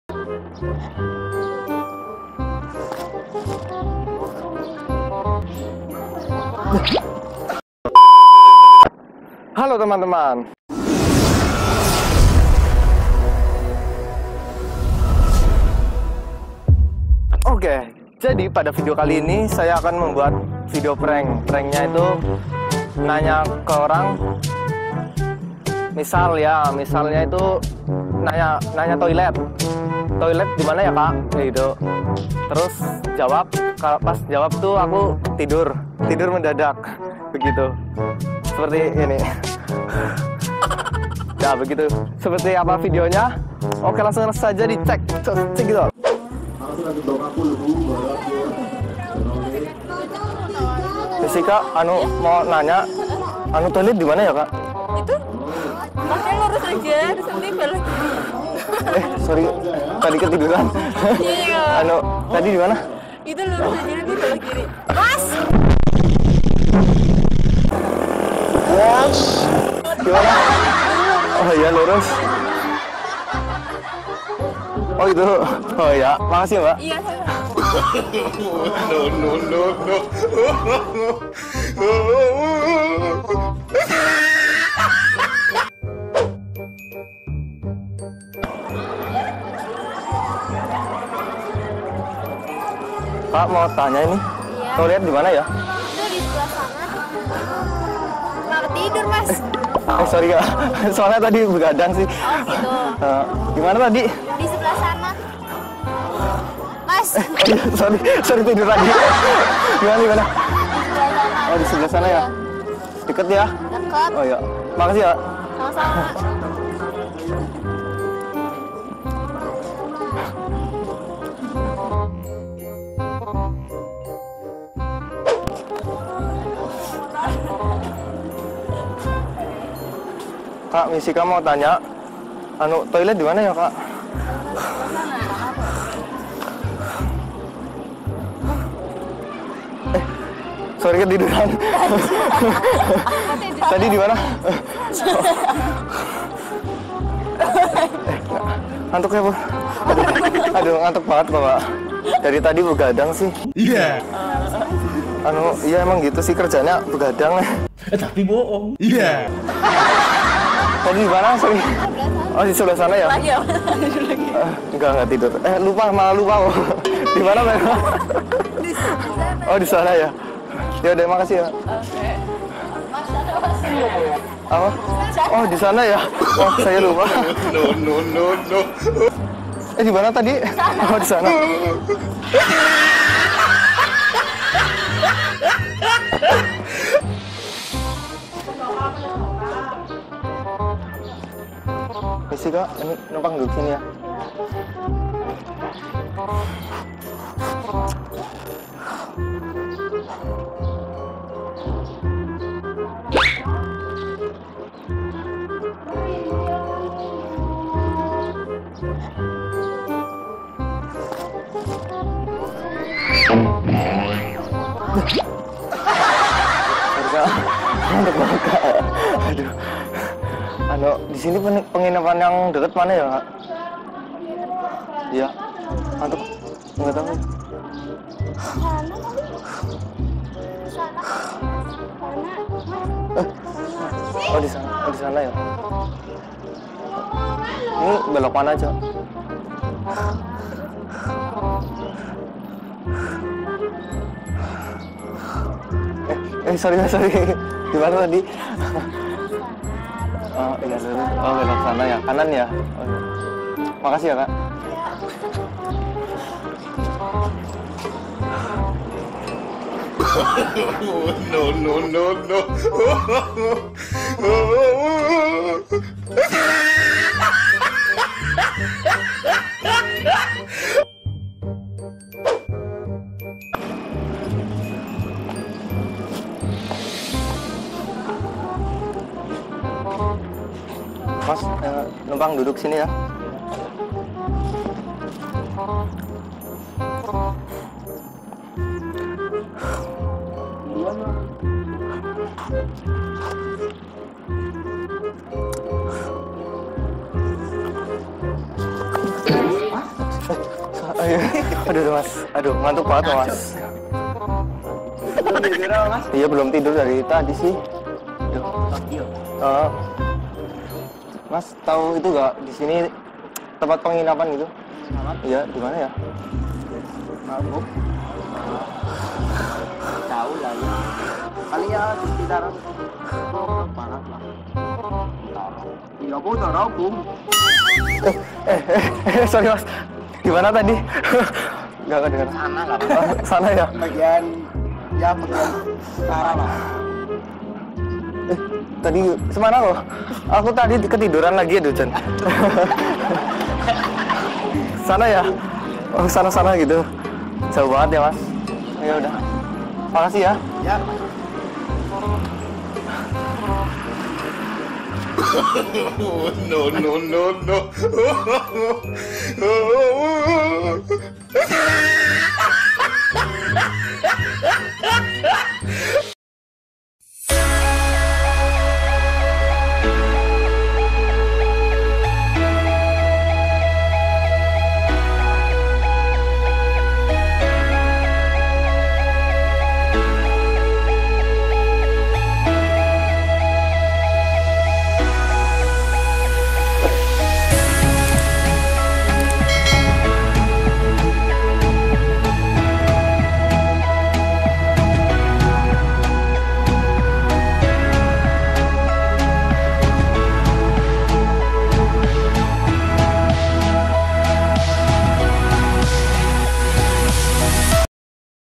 TIDUR DI TEMPAT UMUM!! PRANK INDONESIA!!
TIBA TIBA TIDUR SAAT TANYA!! PRANK INDONESIA!!

Di video ini Julio Nur membuat keseruan kembali dengan tiba tiba tertidur saat tanya ke orang lain,
seperti apa serunya? Yuk tonton video ini..!!!

PRANK TIBA TIBA TIDUR DI TEMPAT UMUM!! PRANK INDONESIA!!